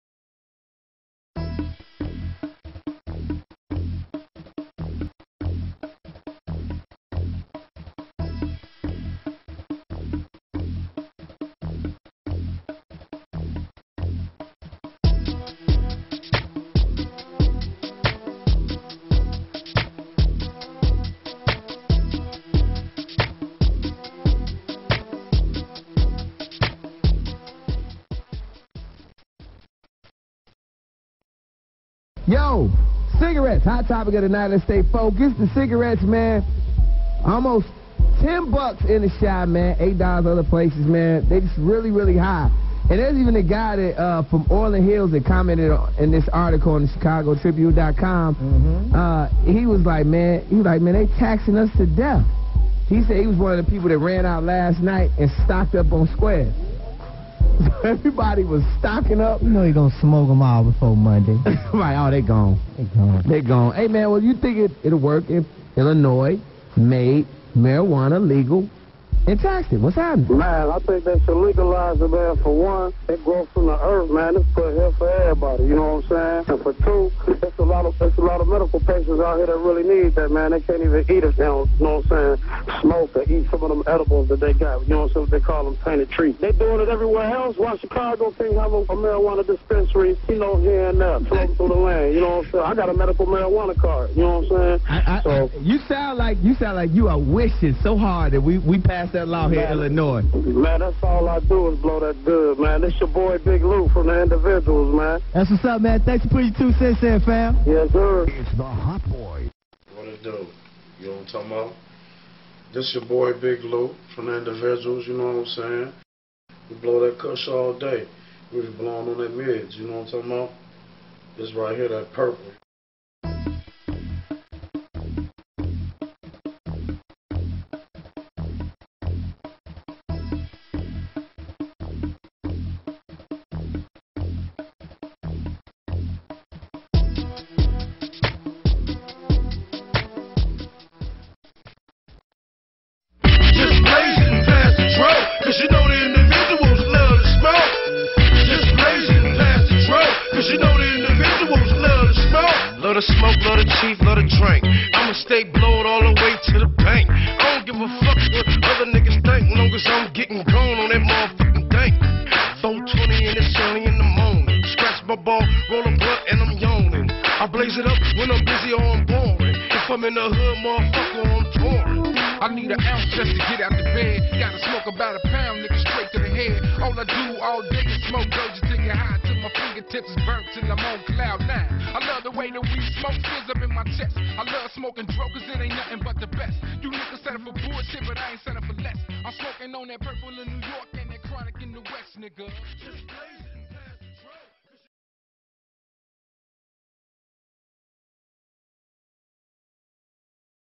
THE ILL STATE UPDATE (Report #4)
On April 1, 2009 the price of cigarettes increased in the State of Illinois to raise more State revenues. Why not Legalize Marijuana?